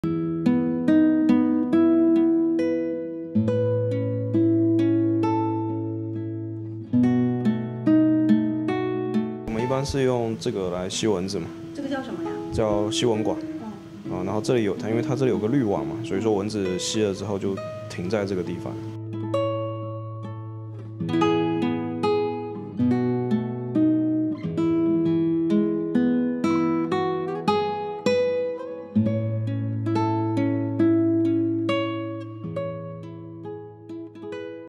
聽說過養貓、養狗，但您聽說過有人養蚊子嗎？背後竟然是為了這個目的．．．
聽說過養貓、養狗，但您聽說過有人養蚊子嗎？在廈門就有這麼一個人專養花蚊子，背後竟然是為了這個目的．．．